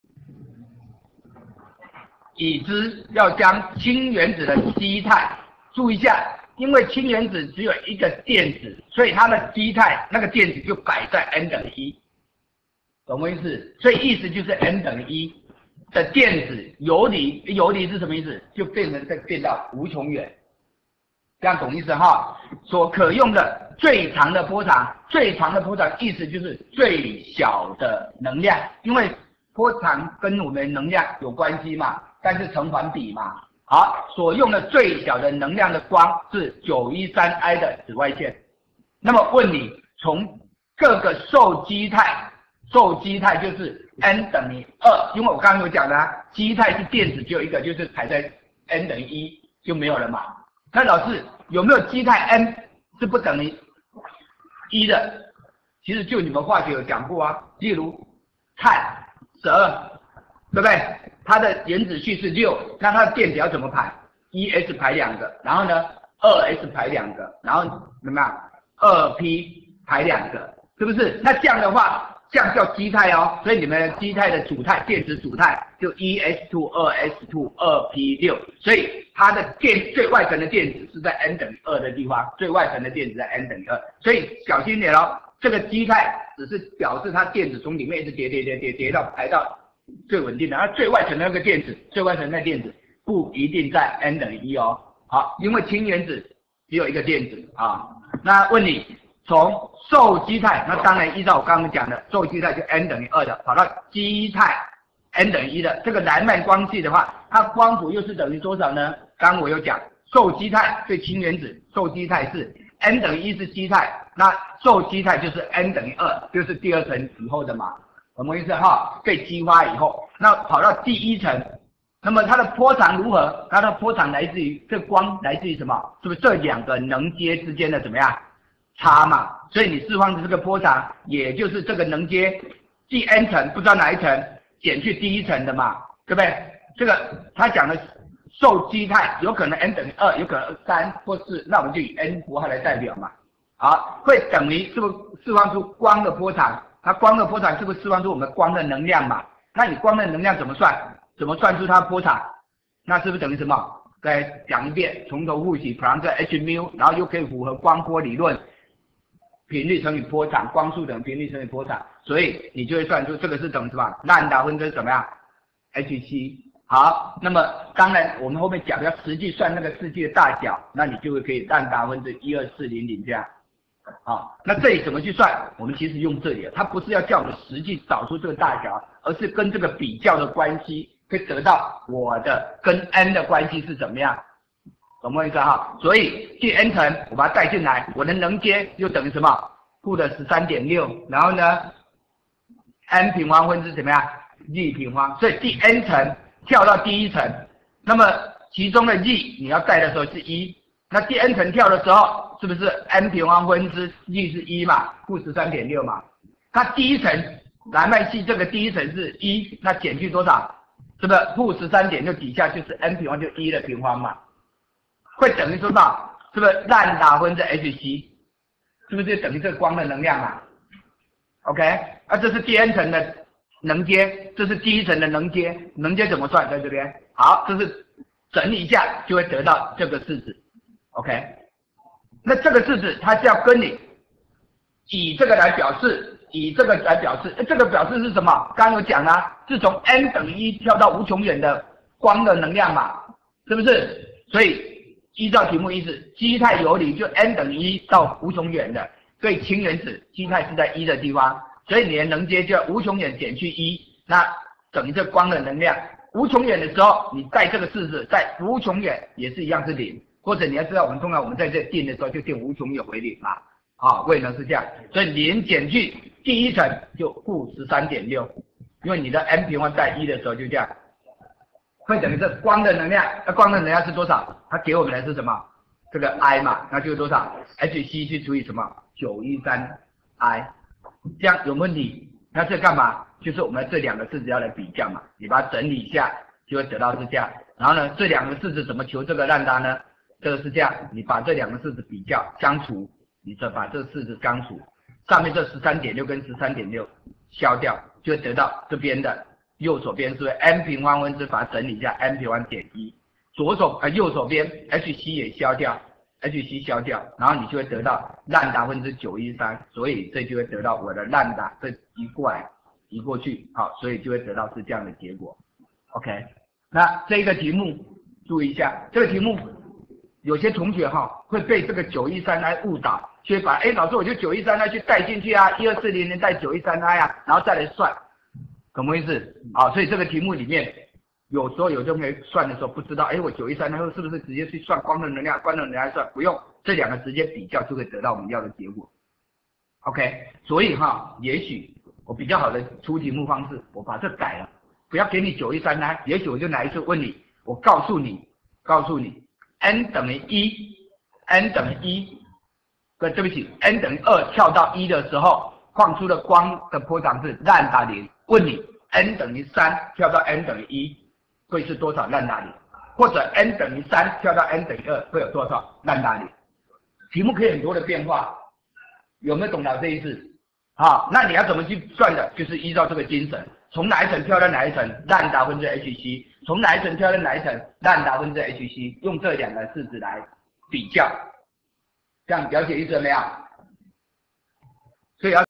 [物理教學影片]範例:能階躍遷與光量子論[72日大]
已知要將氫原子基態的電子游離時,可用的最長波長的光為913埃的紫外光。那麼從各受激態至基態的來曼系光譜,其波長如何表示?(已知量子數為n) [72日大]
其他物理教學相關資源(教學影片,動畫,題庫...等),請參考